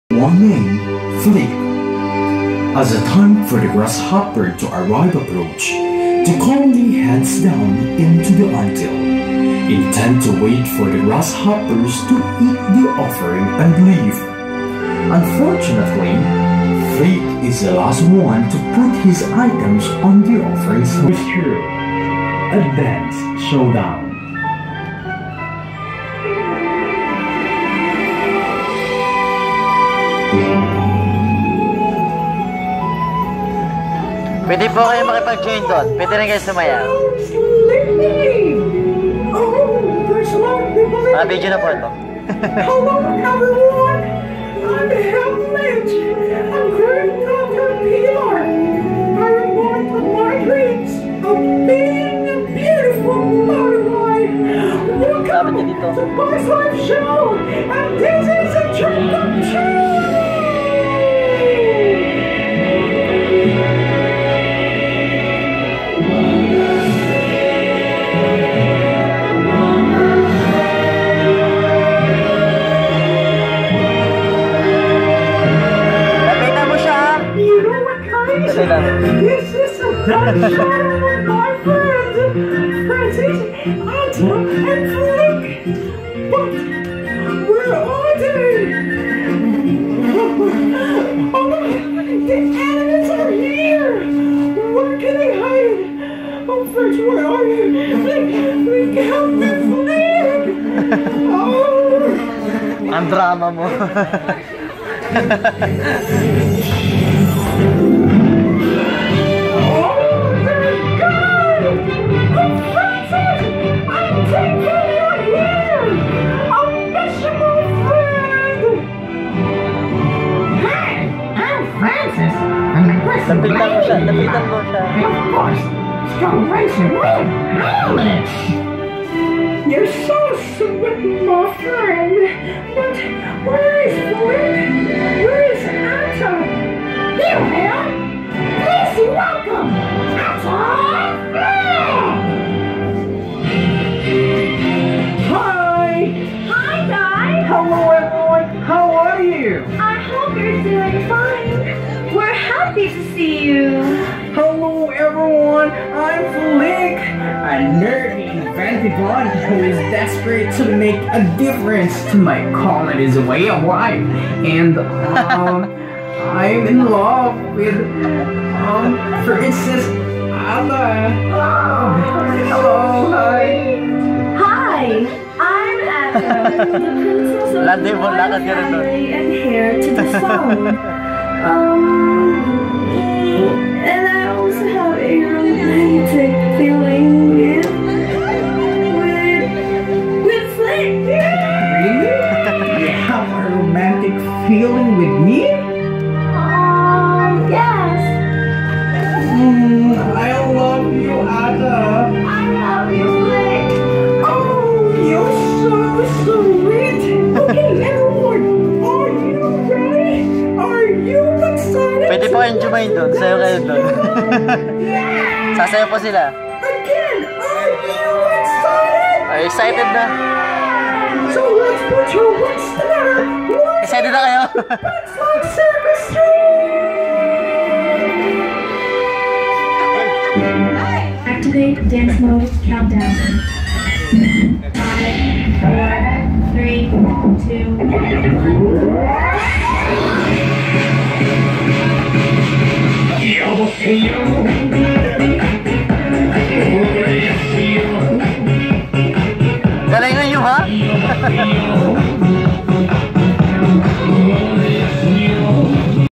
INSECTLANDIA in Clark Land
EXPLORE INSECTLANDIA AND CONQUER THE FASCINATING WORLD OF GIANT INSECTS! Enjoy watching the Bugs life show.